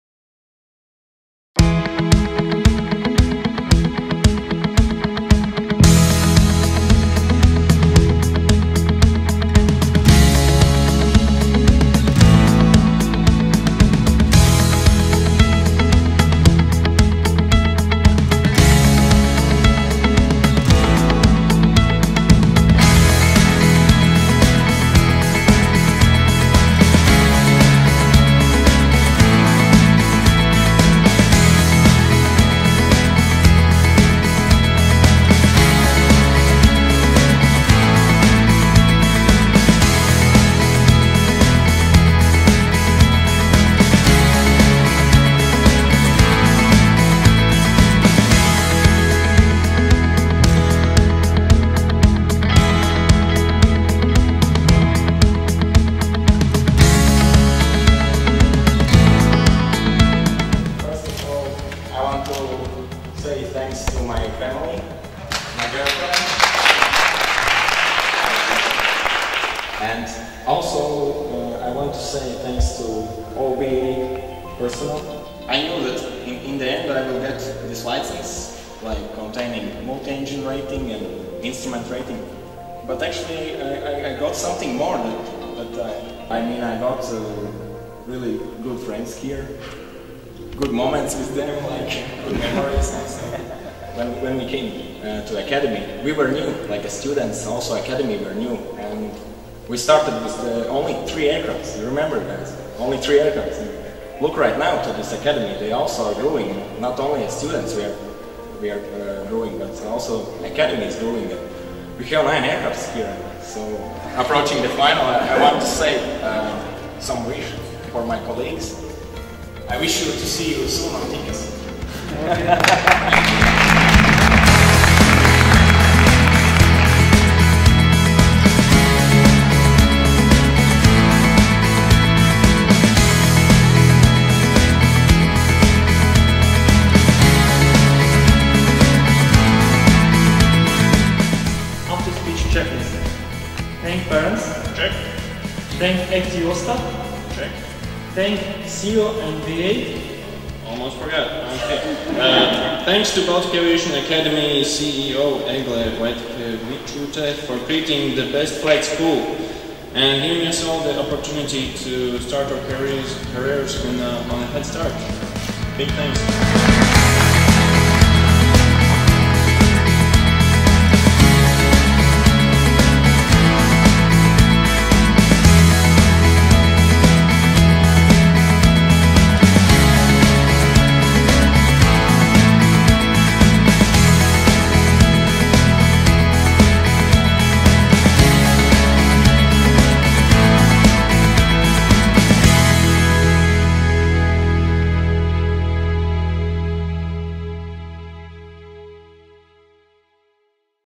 Graduation Ceremony: Class of 2014 - Baltic Aviation Academy - Best New
Baltic Aviation Academy wants to share moments from the graduation ceremony of the class 2014. Students, parents, friends and Baltic Aviation Academy commemorate a lifetime achievement of these students. Baltic Aviation Academy says THANK YOU for a daily inspiration to keep fulfilling more dreams like yours!